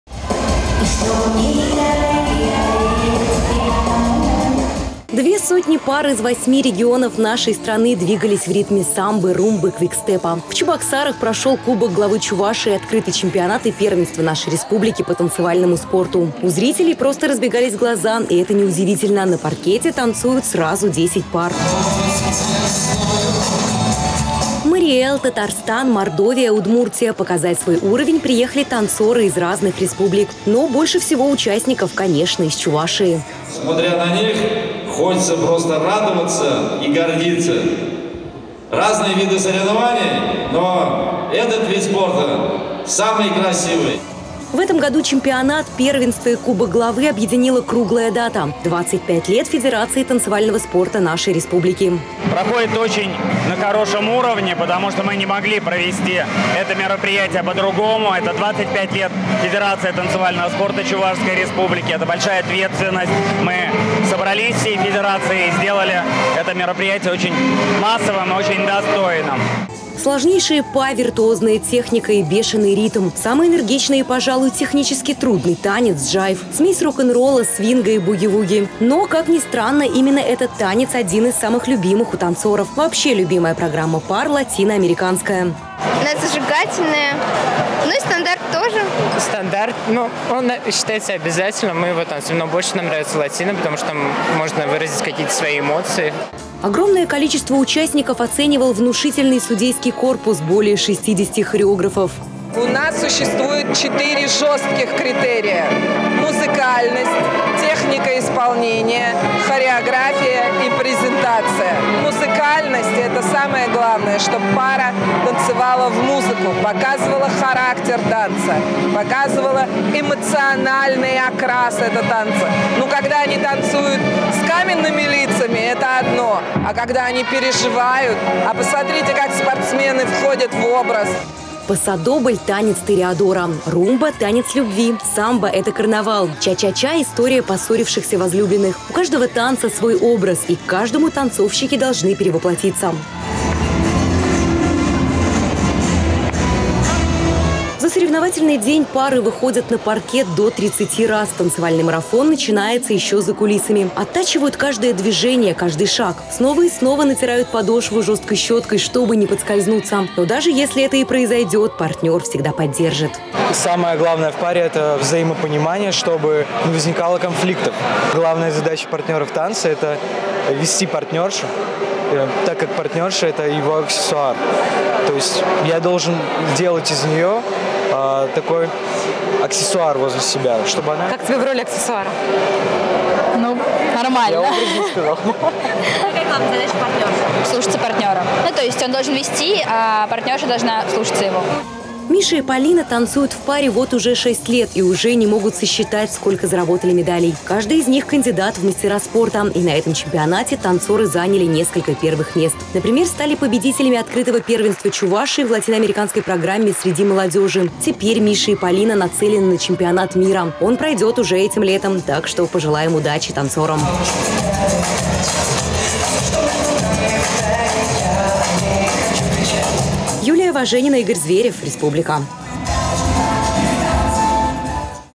В столице Чувашии прошел Кубок Главы Чувашии по танцевальному спорту.
Две сотни пар из 8 регионов нашей страны двигались в ритме самбы, румбы, квикстепа — в столице Чувашии прошел Кубок Главы Чувашии, открытый чемпионат и первенство нашей республики по танцевальному спорту.